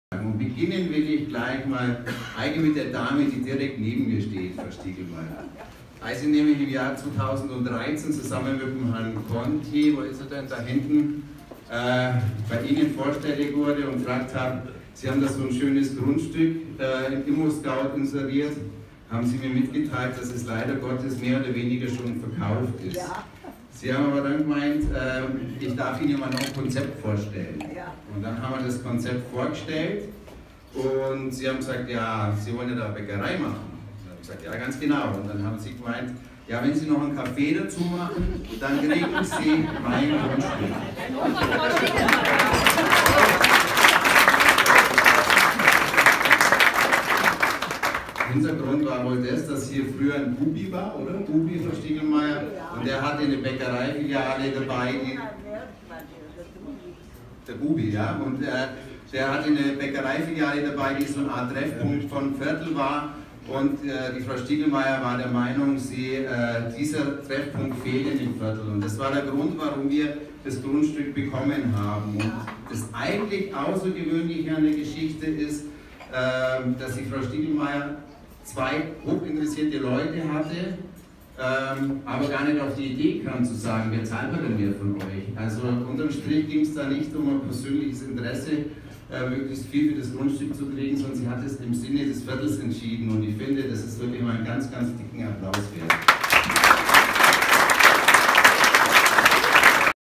So kam die Bäckerei Wolf zum Grundstück der Filiale in Augsburg Hochzoll
Stefan Wolf erzählte in seiner Ansprache über die Hintergründe des Projekts.